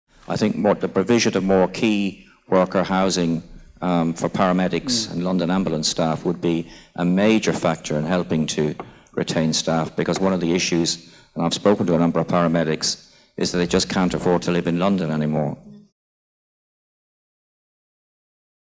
People share their thoughts on the London Ambulance Service - 2
As part of the Health Committee’s investigation into the Future of the London Ambulance Service, Londoners came to a special meeting at City Hall to give their opinions.

The committee wants:

- To examine the key challenges facing the performance of the London Ambulance Service.

-To establish how the Mayor and Greater London Authority group can further support the London Ambulance Service to provide a more modern, efficient and effective emergency service for all Londoners.

- To examine ways of making the ambulance service more open and accountable to the people of London.